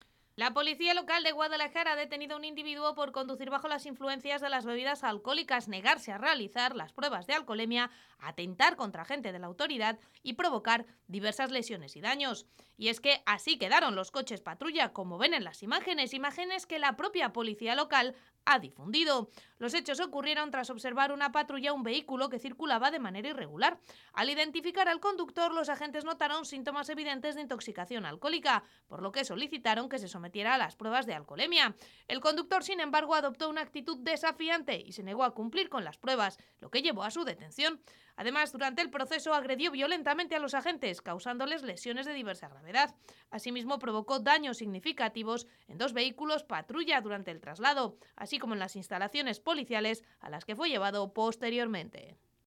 Arremete contra la Policía Local por querer hacerle un control de alcoholemia
La Policía Local detectó a un conductor circulando de manera peligrosa y el conductor arremete contra la los agentes y el coche patrulla.